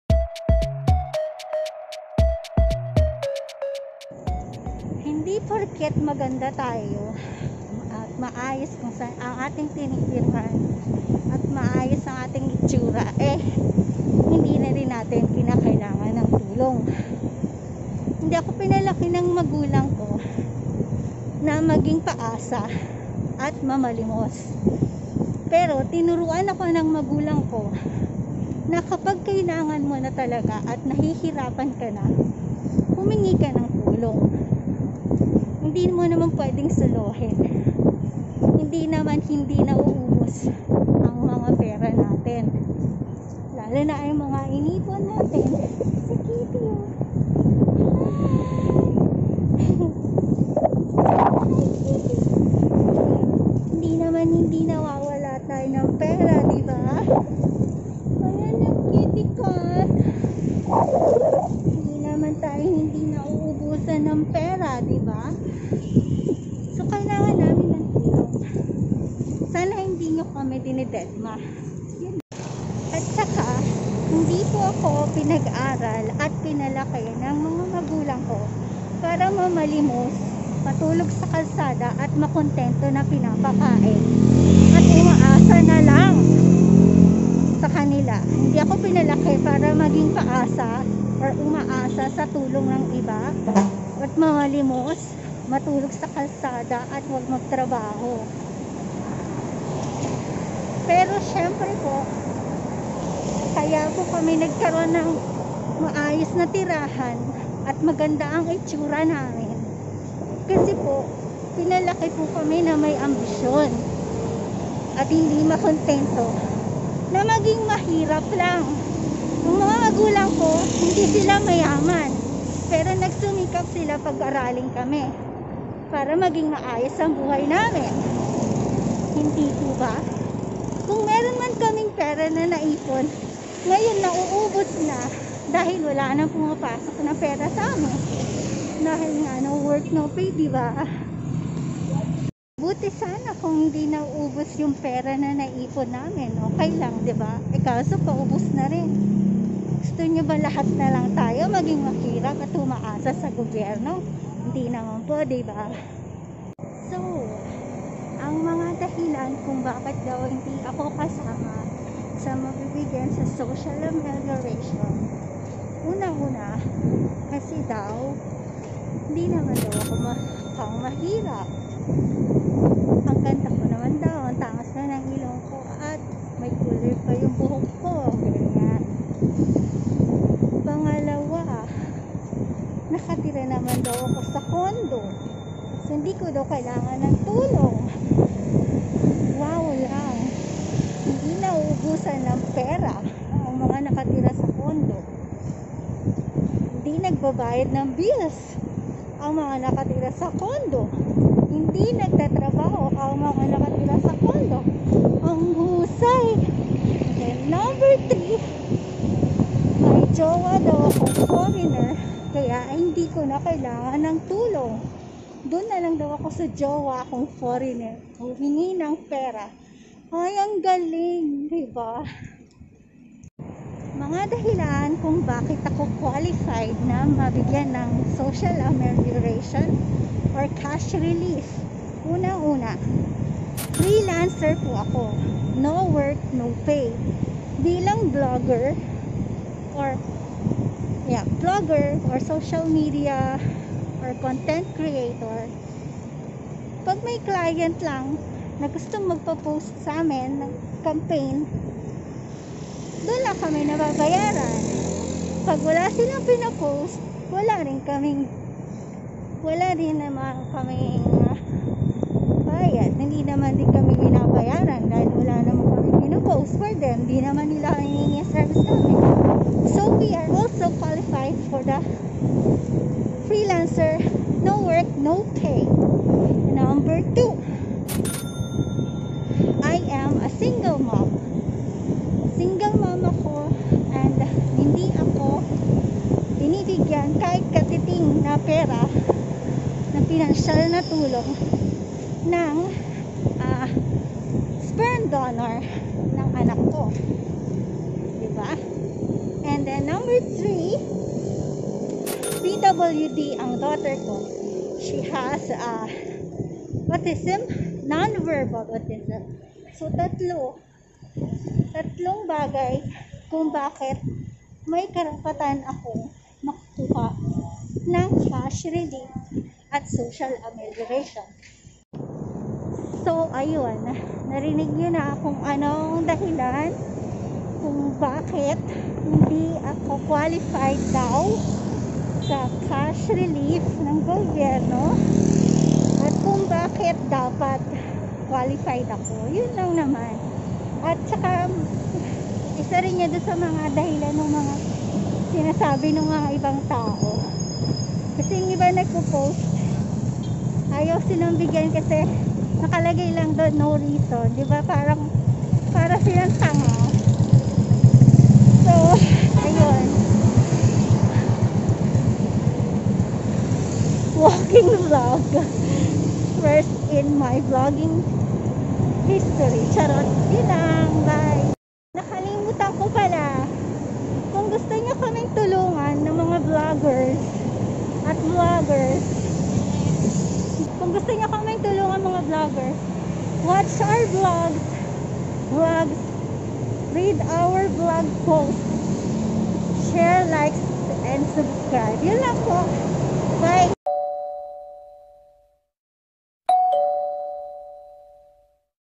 Long Vent: Bakit Hindi ka Qualified bigyan Ng Cash Relief (SAP) ng Gobyerno
Long Vent: Bakit Hindi ka Qualified bigyan Ng Cash Relief (SAP) Social Amelioration Program ng Gobyerno at Paano tulungan Ang mga blogger / vlogger / freelancers during this trying times of Lockdown